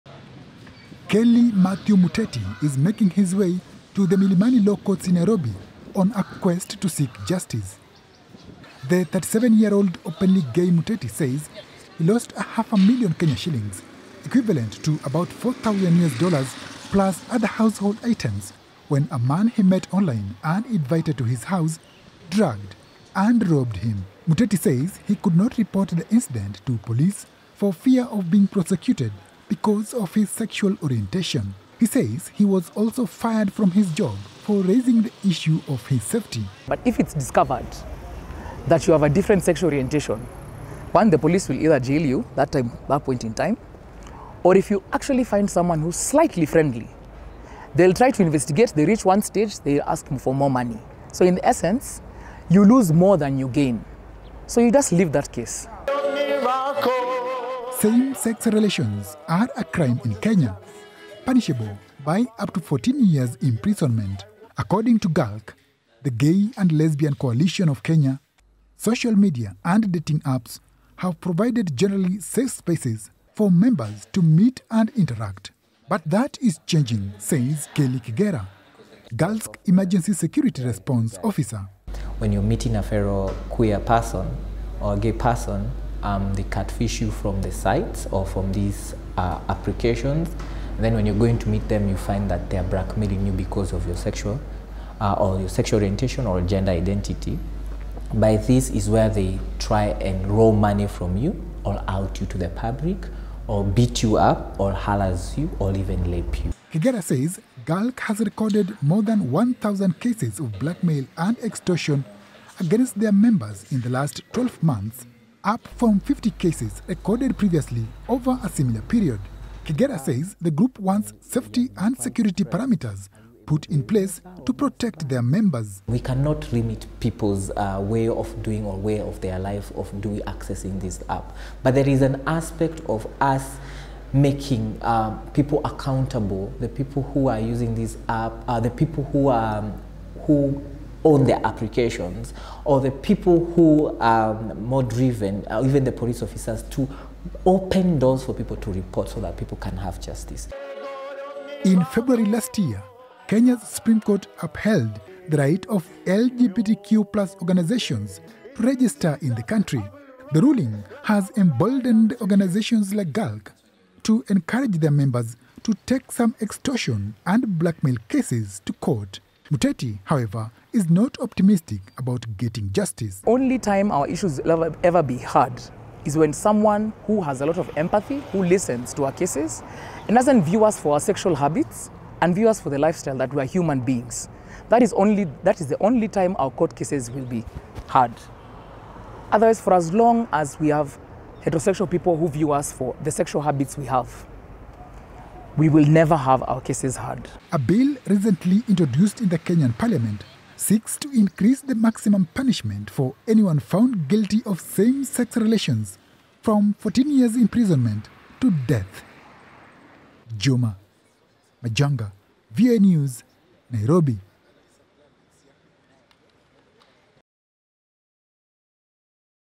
LGBTQ+ Kenyans decry surging blackmail, extortion on dating apps | VOANews
In Kenya, where same-sex relations constitute a crime, the only way the LGBTQ+ community can meet is through dating apps and social media. Now the platforms are being used to trap victims in a web of blackmail, extortion and assault. Juma Majanga reports from Nairobi, Kenya. Camera: Amos Wangwa.